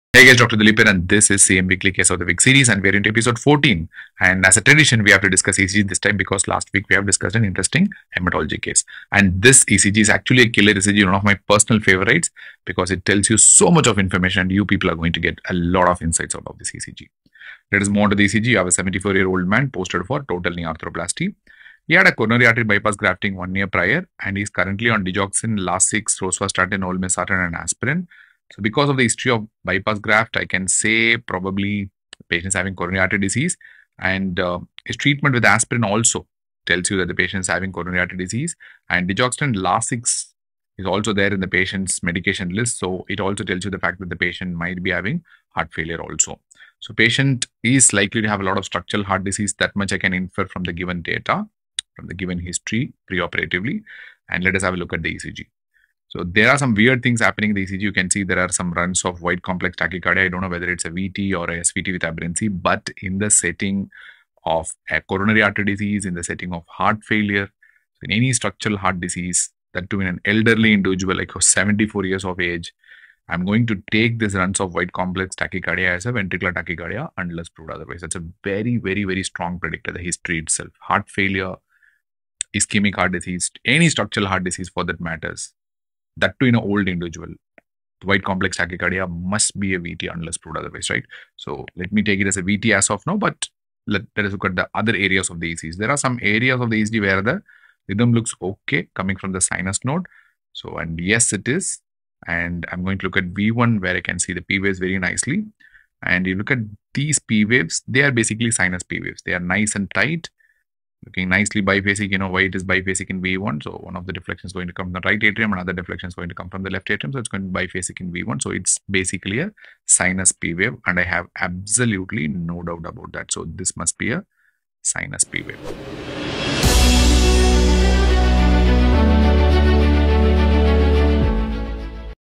Case of the Week- Episode 14 | Dr. Dilip Kumar
Welcome back to the Case of the Week series with Dr. Dilip Kumar! In Episode 14, Dr. Dilip walks you through a high-yield and thought-provoking ECG case that every medical student and practitioner should understand.
Don't miss this must-watch video for all Medicine Residents.

For Special Offers Connect with CM Helpline Nos:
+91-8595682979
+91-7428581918